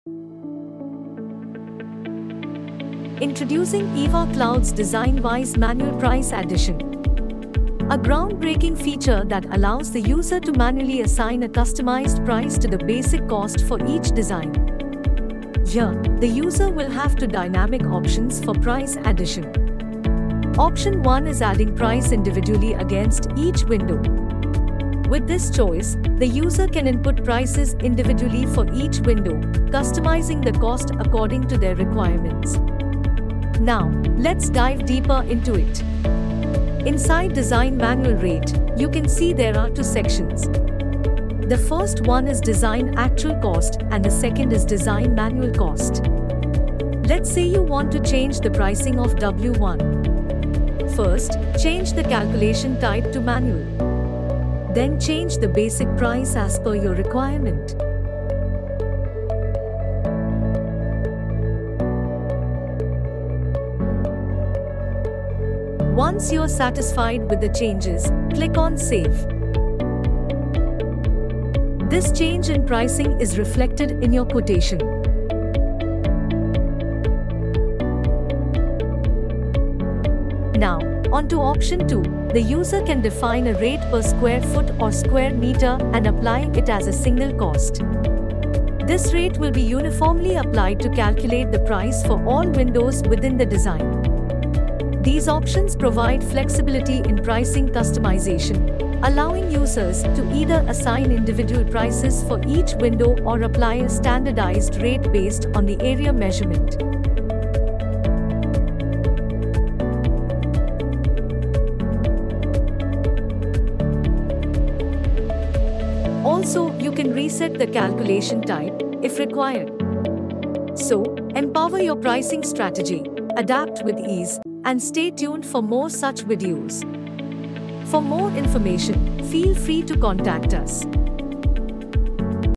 Introducing Design-Wise Manual Price Addition Feature | Window and Door Quoting Software
🚀  A groundbreaking feature that allows the user to manually assign a customized price to the basic cost for each design.

🔀 Here, the user will have two dynamic options for price addition.

1️⃣  Adding Price Individually against Each Window
2️⃣ Defining Rate per (SQFT) and Rate per (SQMT) by Applying a Single Cost

🔗 KEY LINKS